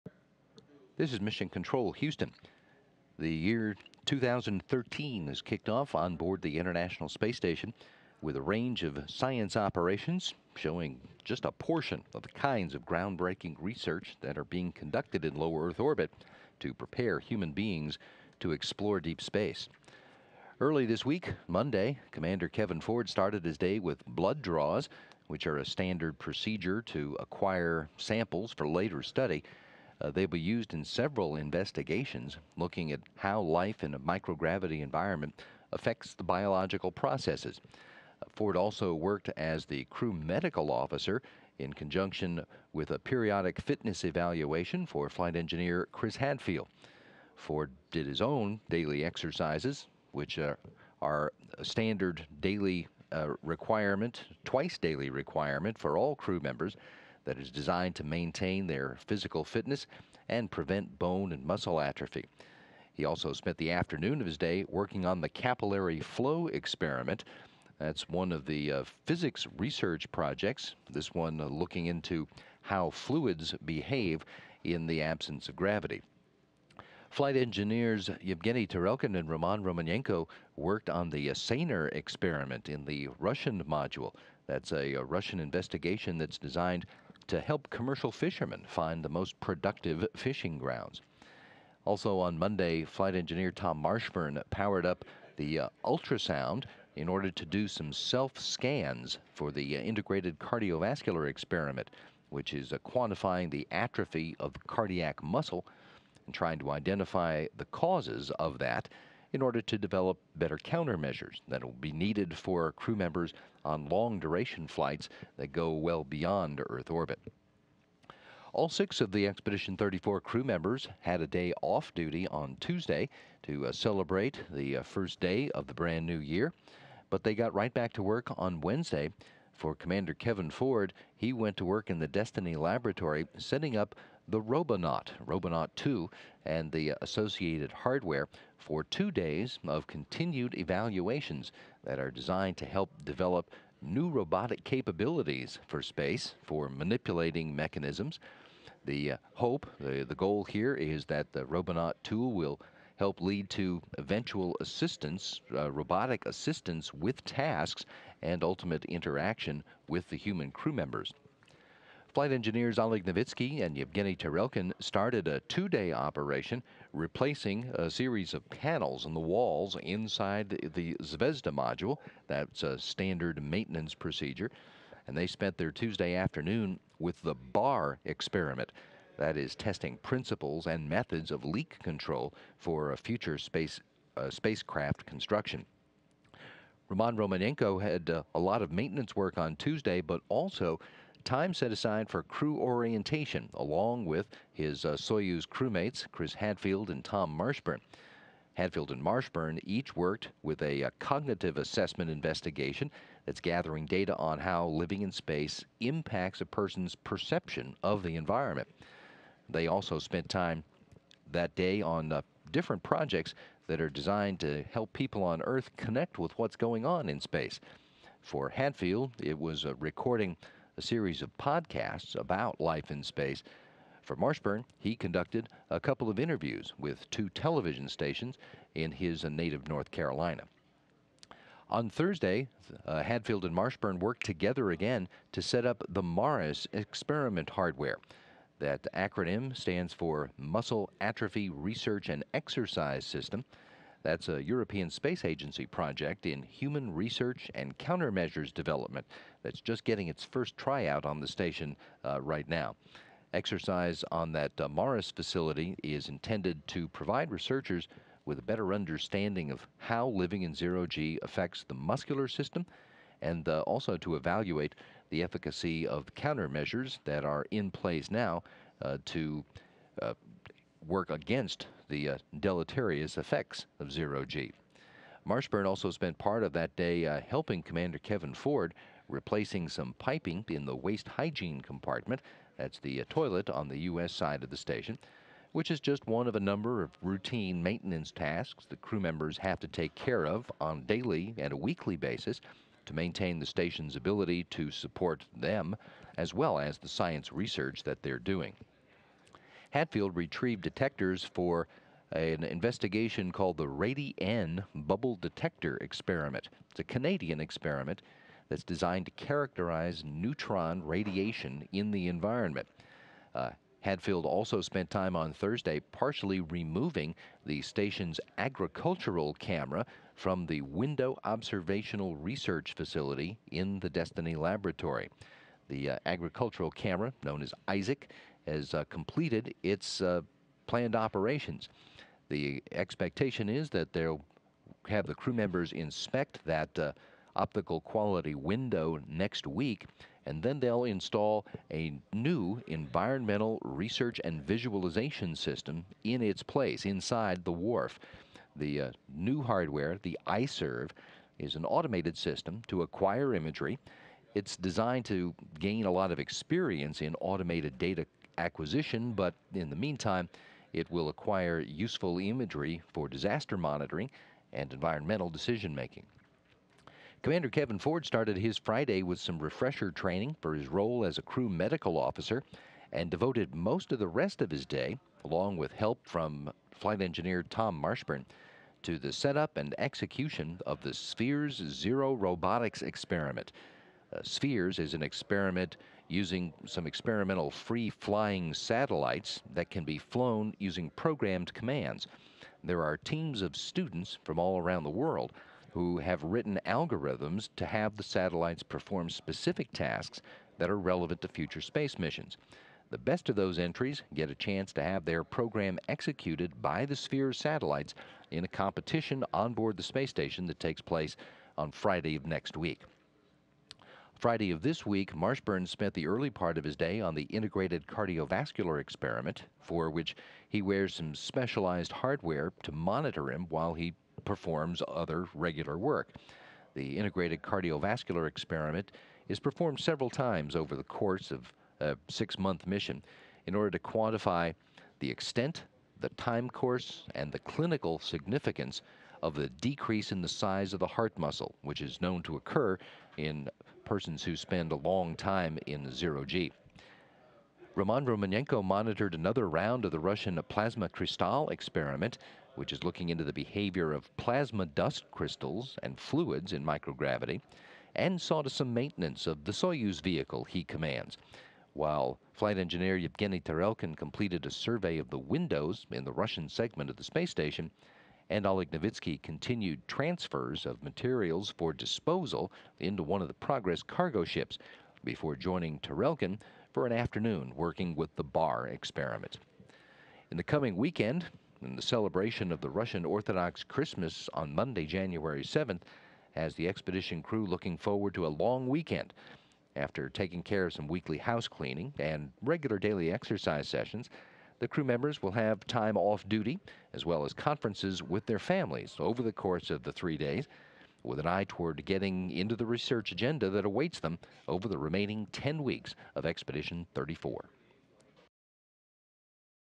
ISS Update: Weekly Recap for Jan. 4, 2013
The International Space Station recap video for the week of Dec. 31 2012 - Jan. 4, 2013.